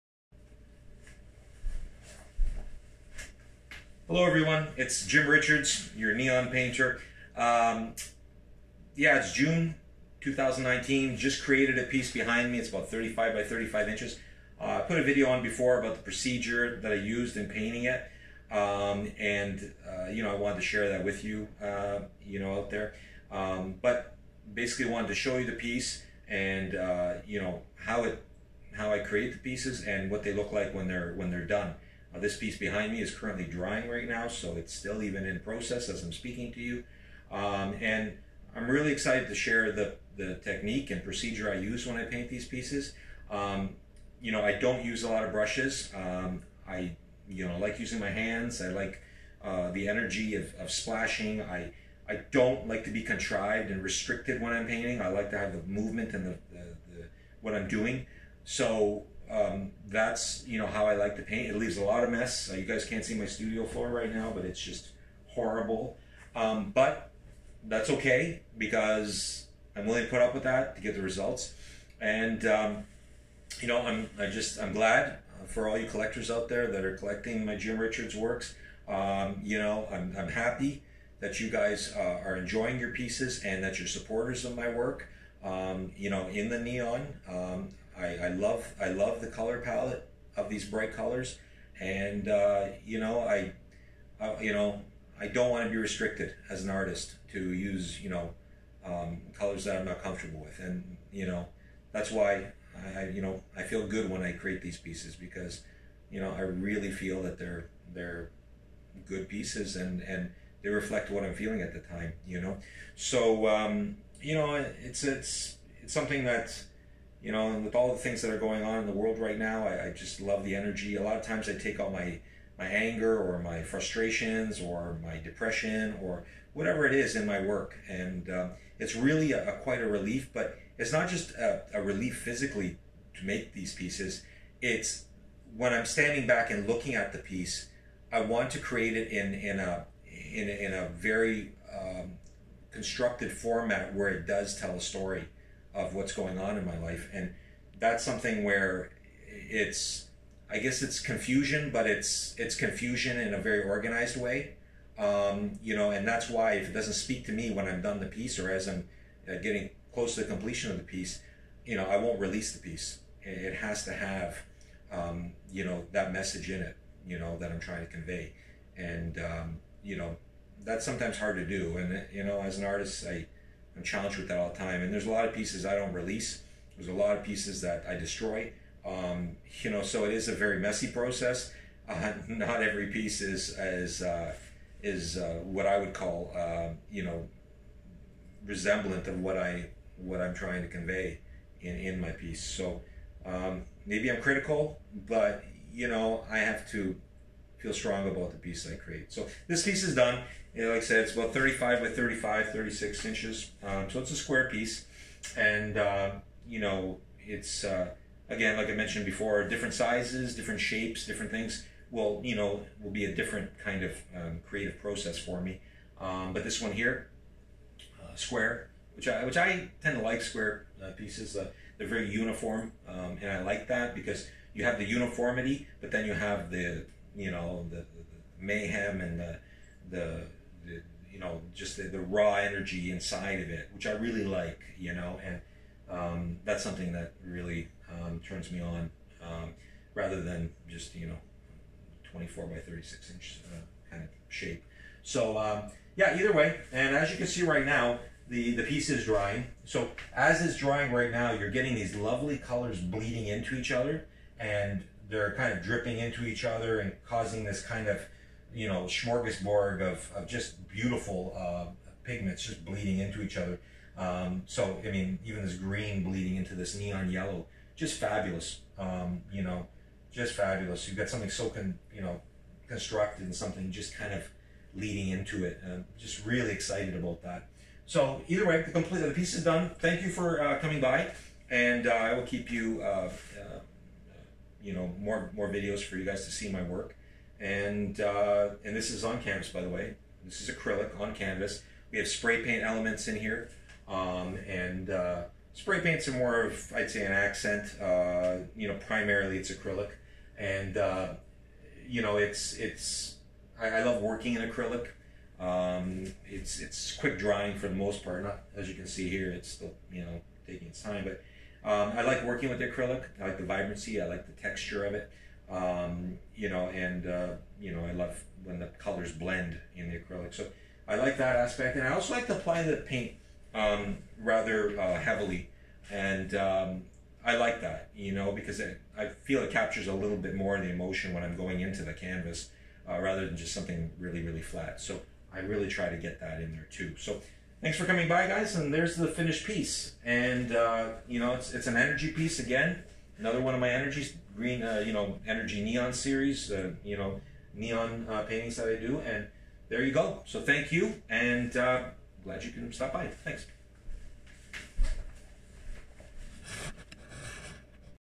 Jim Richards shows how to paint his abstract expressionist modern art painting in neon colors
Also Saatchiart.com search "Jim Richards"